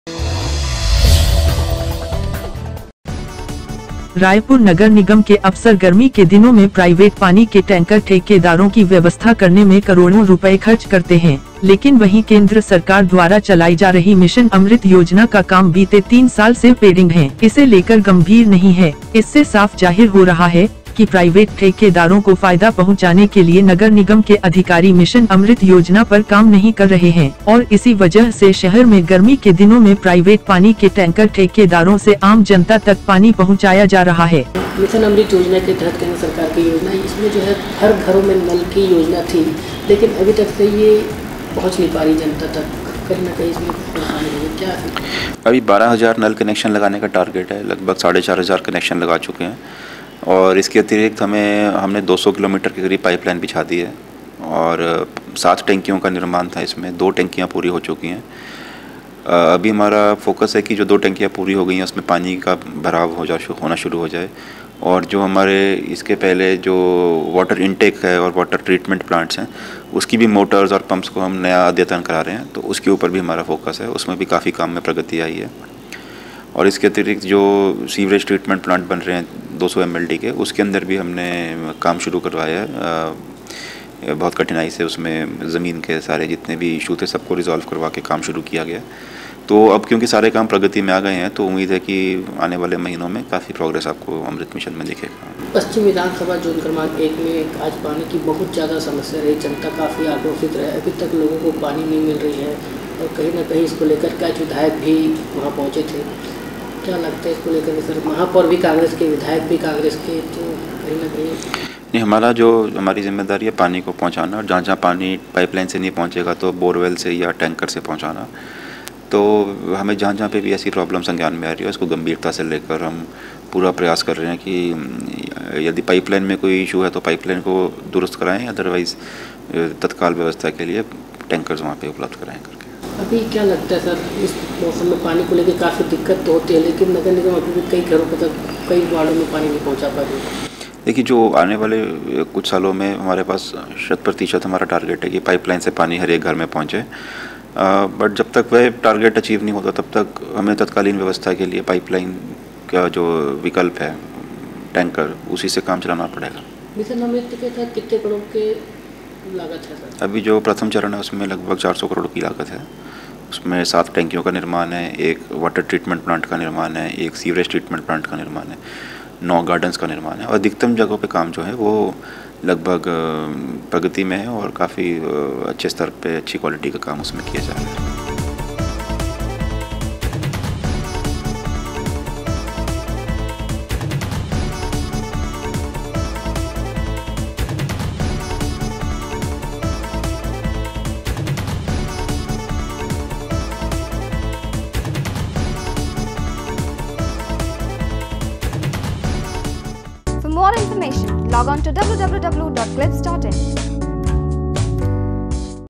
Video : निगम अफसर प्राइवेट ठेकेदारों को फायदा पहुंचाने मिशन अमृत योजना पर लगा ब्रेक
नगर निगम प्राइवेट पानी के टैंकर ठेकेदारों की व्यवस्था करने में करोड़ों रुपए खर्च करते हैं। लेकिन वहीं केंद्र सरकार द्वारा चलाई जा रही मिशन अमृत योजना का काम बीते तीन साल से पेडिंग हैं, इसे लेकर गंभीर नहीं है। इससे साफ जाहिर हो रहा है कि प्राइवेट ठेकेदारों को फायदा पहुंचाने के लिए नगर निगम के अधिकारी मिशन अमृत योजना पर काम नहीं कर रहे हैं और इसी वजह से शहर में गर्मी के दिनों में प्राइवेट पानी के टैंकर ठेकेदारों से आम जनता तक पानी पहुंचाया जा रहा है।